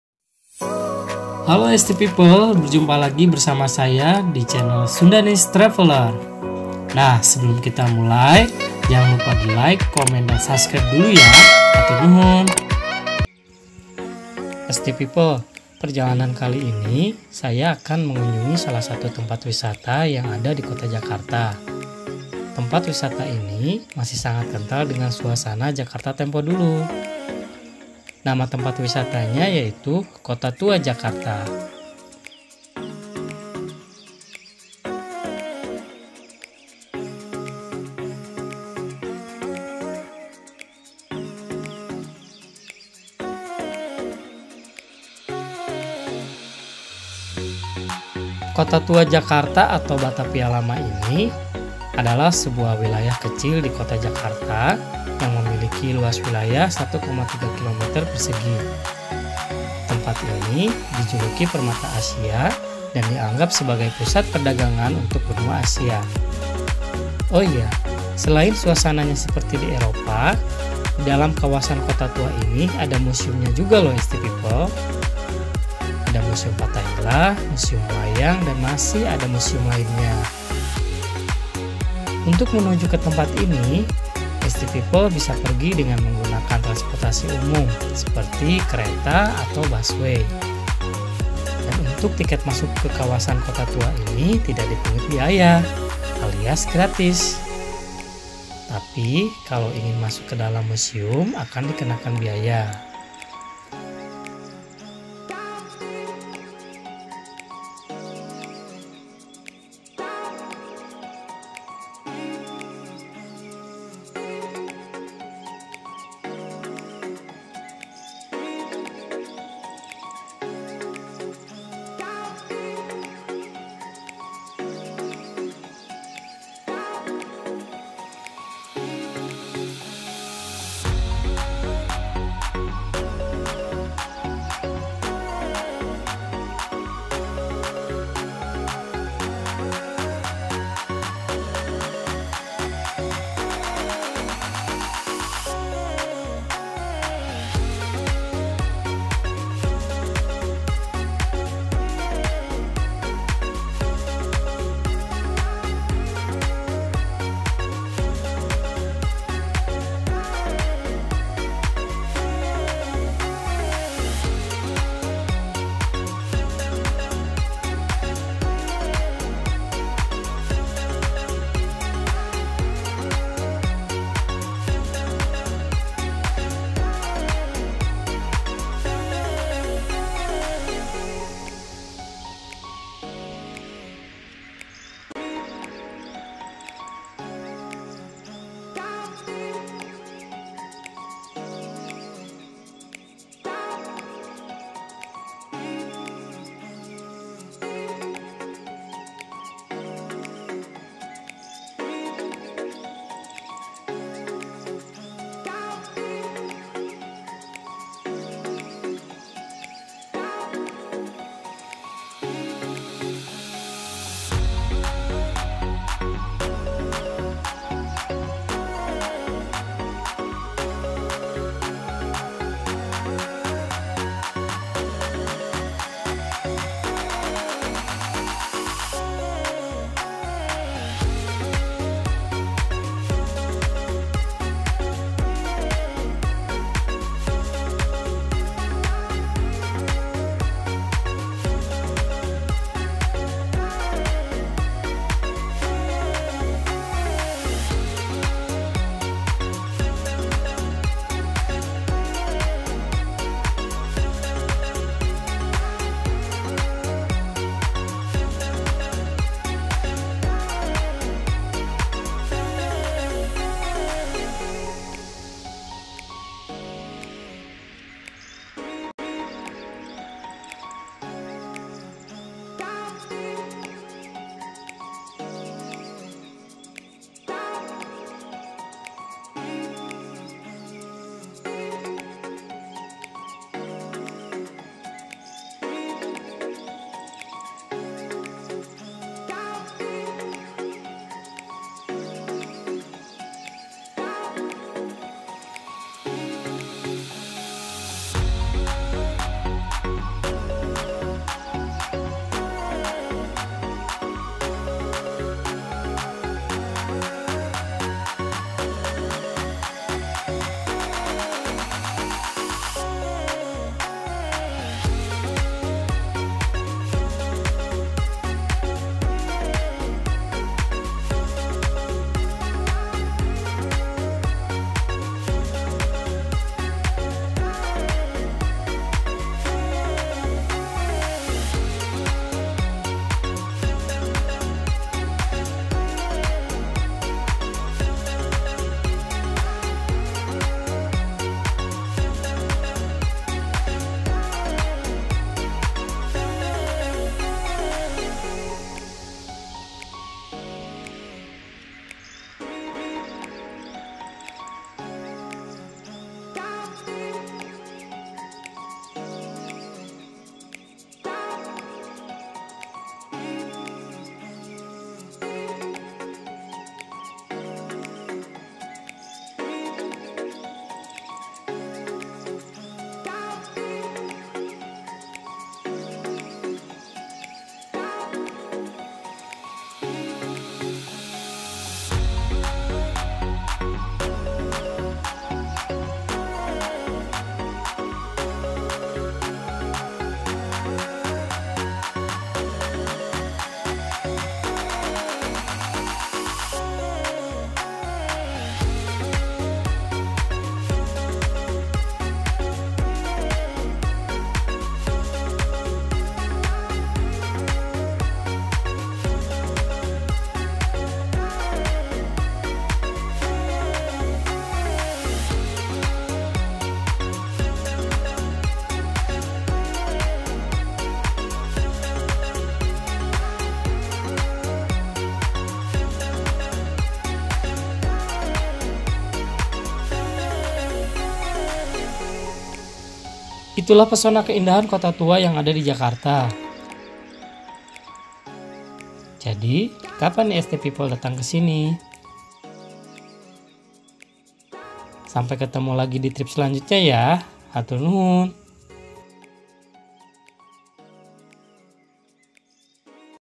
Jelajah Wisata Kota Tua Terkini | SUNGAINYA BERSIH BERASA DI EROPA | vlog traveller
SUNDANESE TRAVELLER Jelajah Wisata Kota Tua Jakarta Terkini | SUNGAINYA BERSIH BERASA DI EROPA | vlog traveller Halo ST People vlog traveller kali ini, saya mau mengajak ST People untuk jelajah wisata kota tua jakarta dan menurut informasi sekarang sungainya bersih seperti berasa di eropa, penasaran videonya tonton terus ya dan dukung terus chanelnya

Jelajah Wisata Kota Tua Jakarta Terkini | SUNGAINYA BERSIH BERASA DI EROPA | vlog traveller